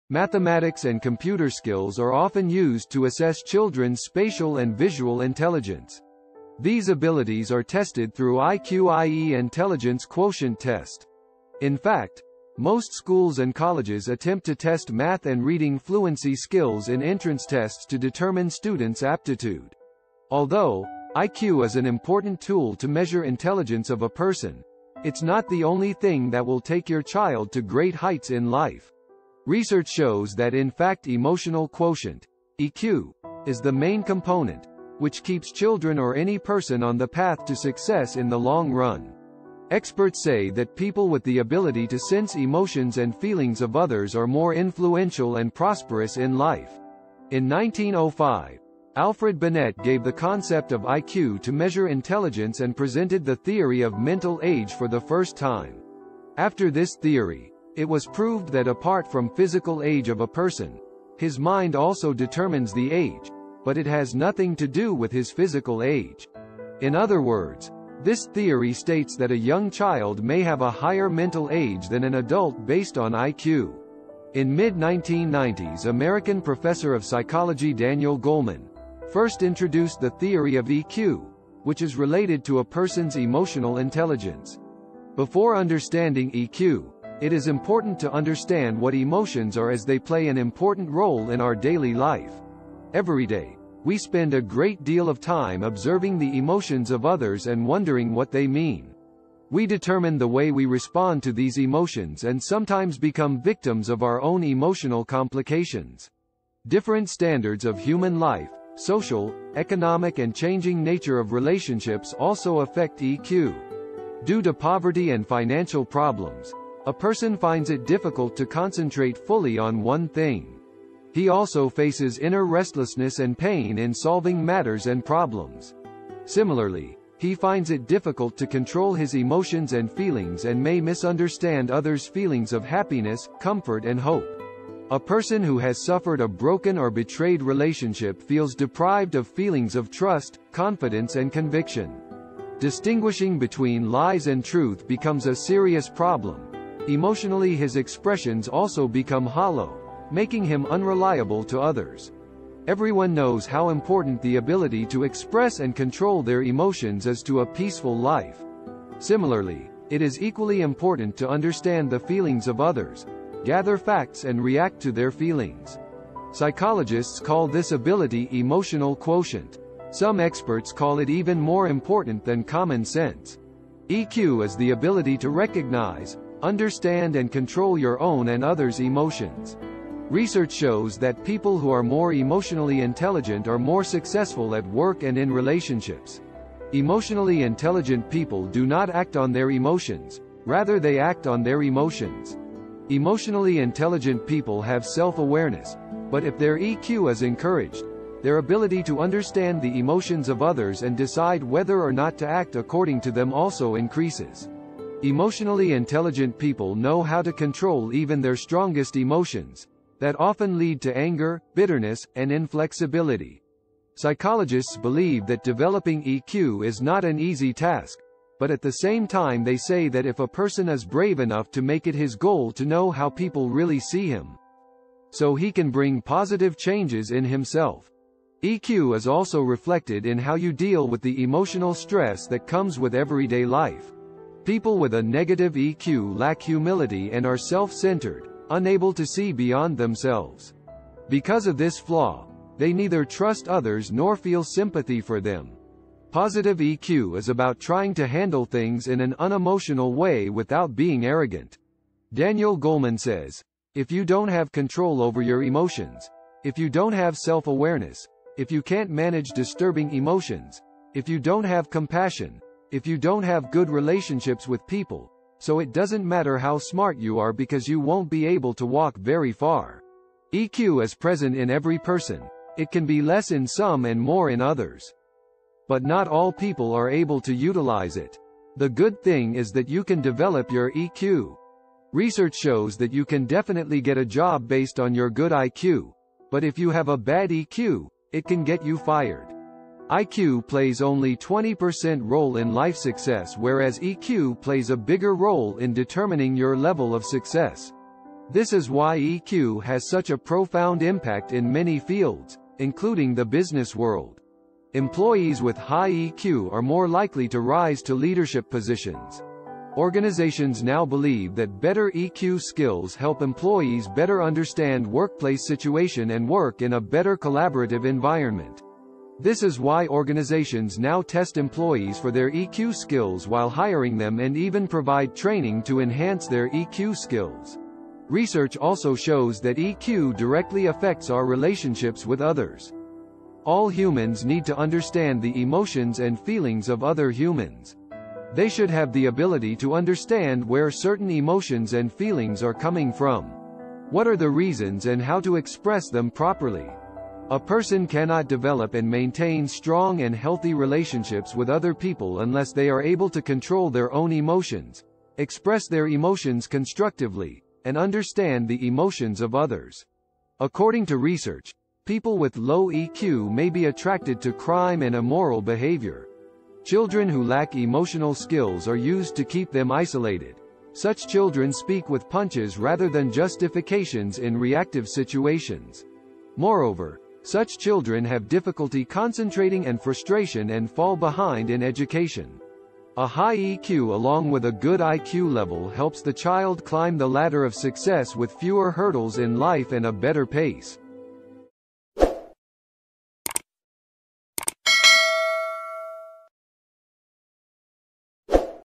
How Emotional Intelligence Work| Solution of All Problems | Life Saver | Motivational Video
A short video about how to work on life problems with Emotional Intelligence j including financial issues, relationship problems, career problems and health problems.
Emotions play a very critical role in the overall quality of our personal and professional lives, more critical even than our knowledge and experience. While technology can help us to learn and master information, nothing can replace our ability to understand our emotions and the emotions of those around us.